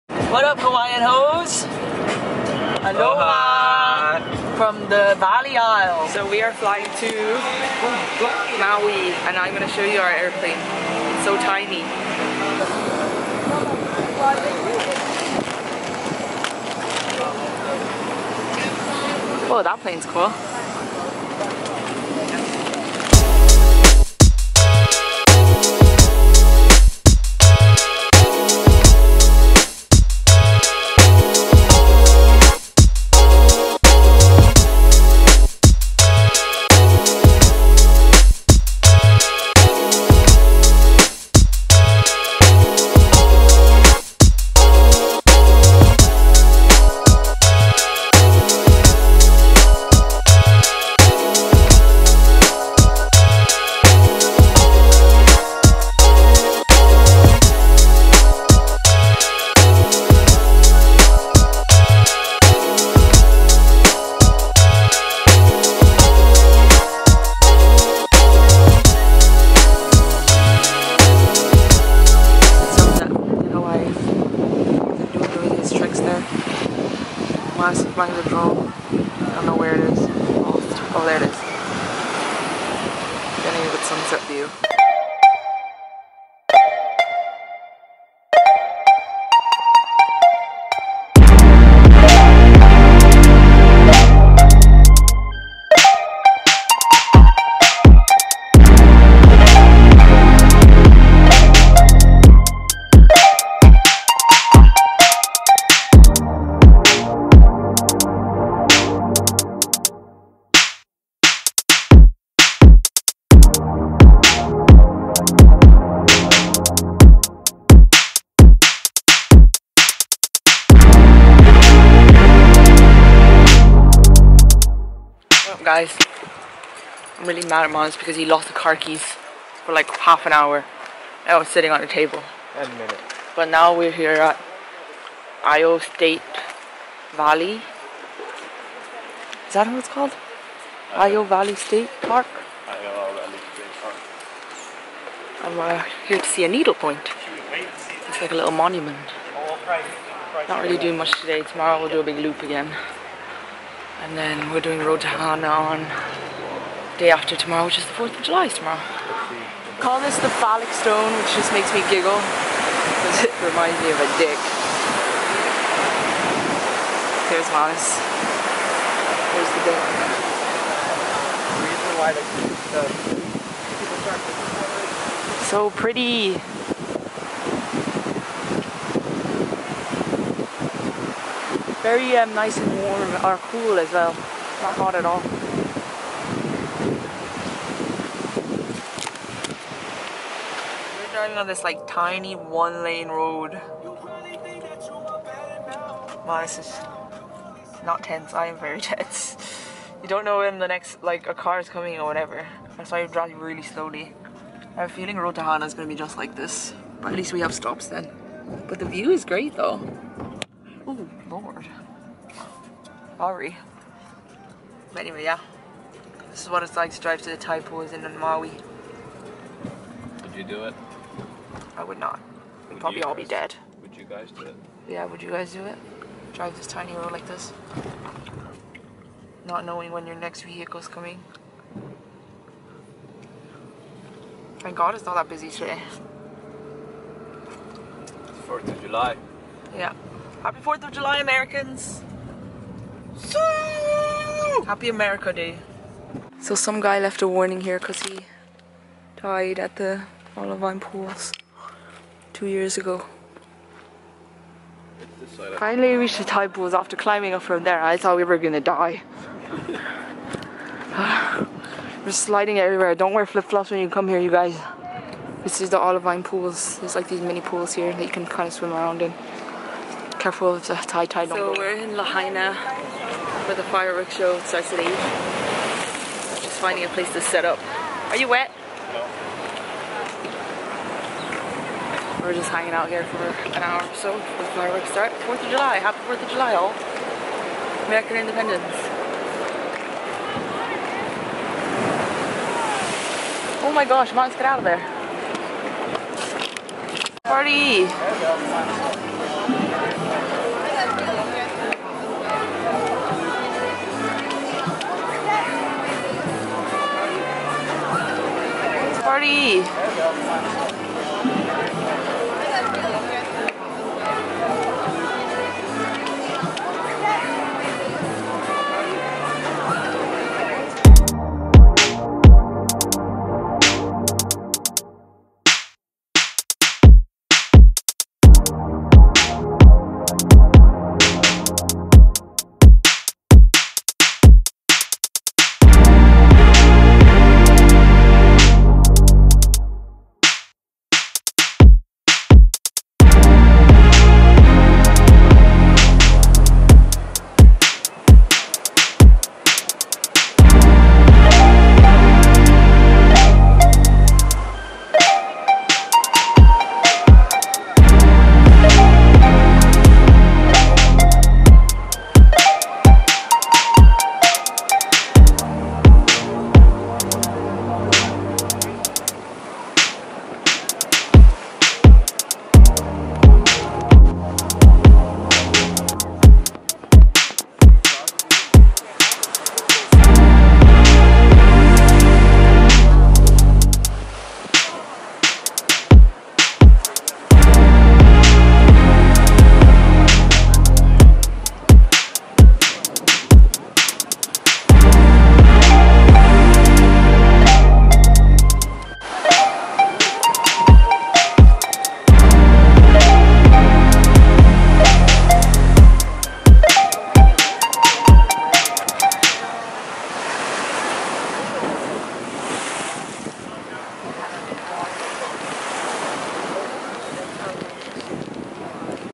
SUNSETS IN MAUI, HAWAII - DAY 1
Hi all - sorry for the delay in the upload, we have been extremely busy traveling ;) but we promise to be more frequent with our uploads. Here is our first day in Maui - what a magical island!